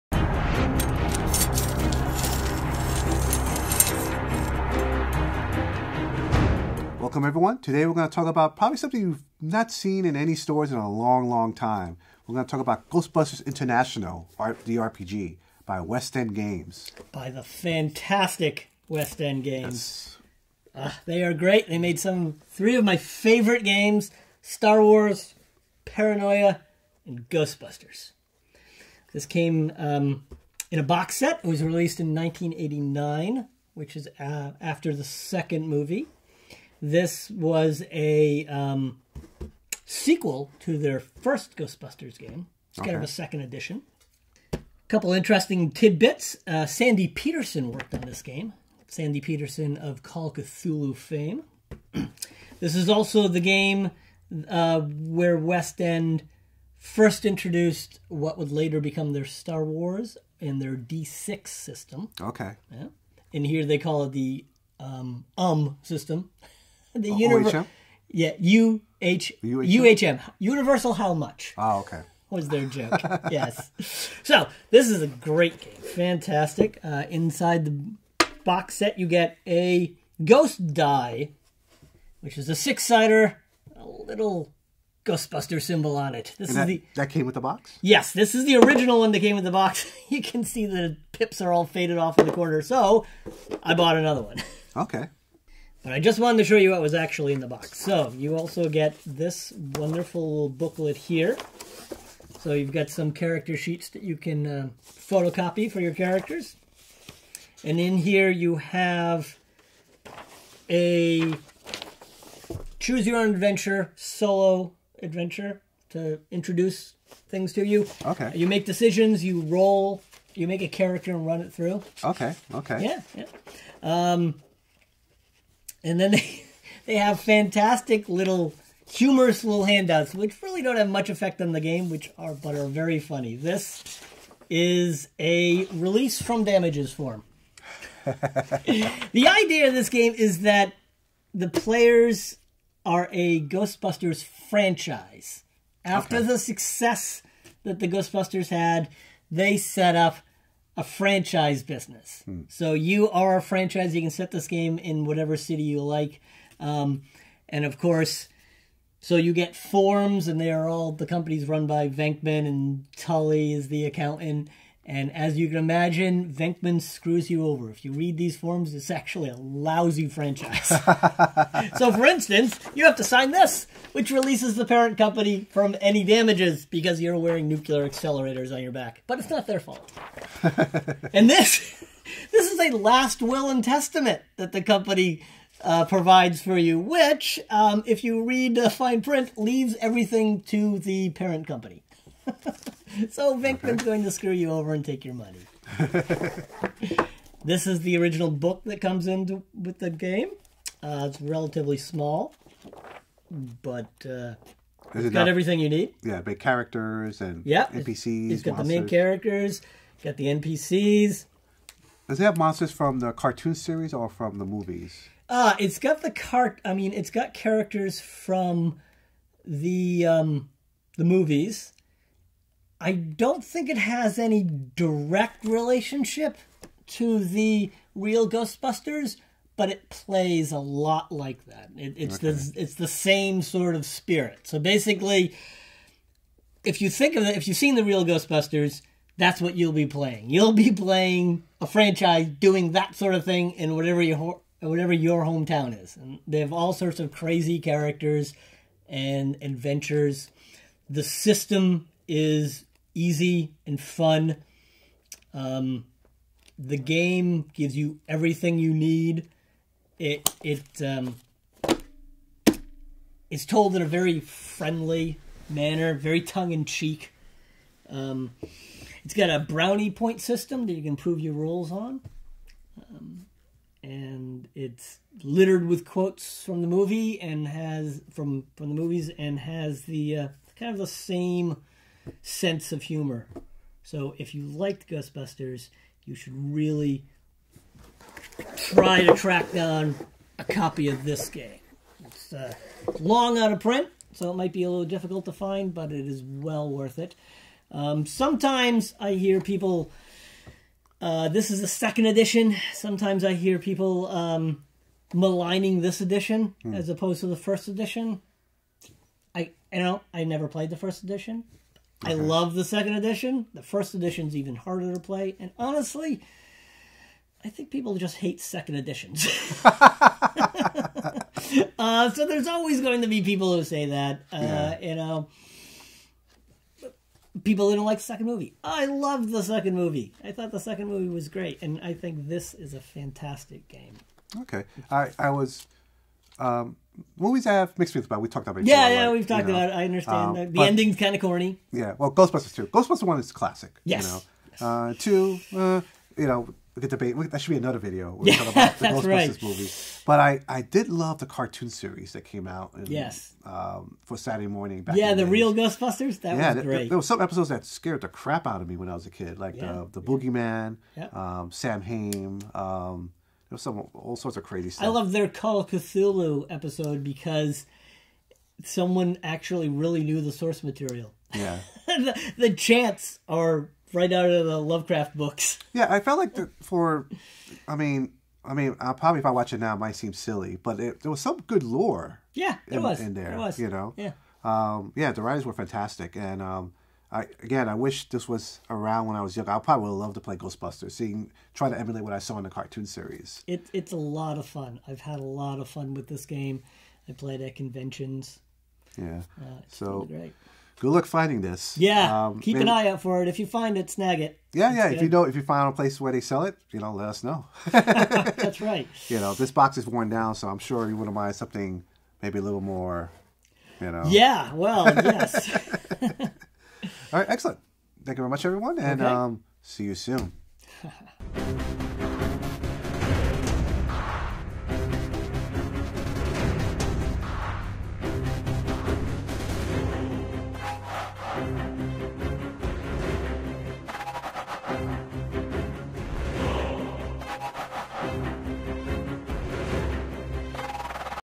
Ghostbusters International From West End Games Is Still A Great RPG
Matt shares with us about his love for West End Games' Ghostbusters International, a roleplaying game based on the popular Ghostbusters movies.